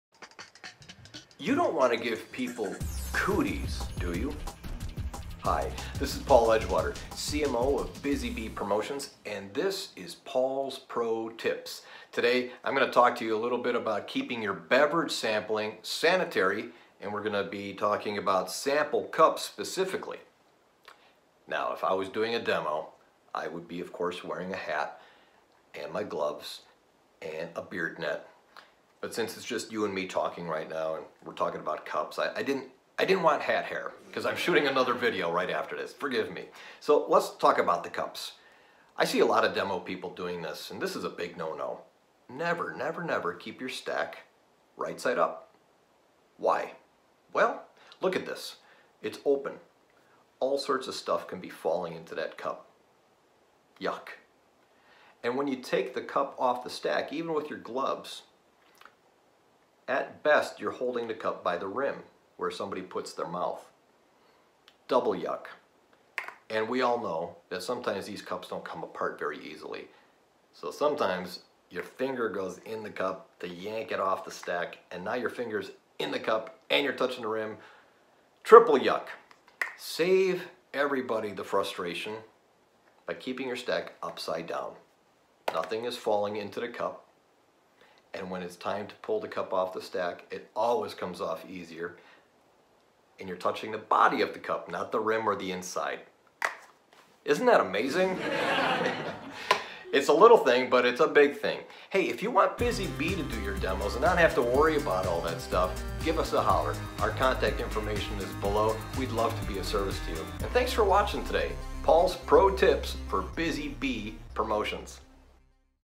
Oh, no!! Cooties!! Paul's Pro Tips: Sample Cup Sanitation
Busy Bee Promotions' very own Paul Edgewater, shares an ancient secret to keep away cooties during your demos! A best-practice in food handling!

Have Busy Bee manage and execute your in-store demo program!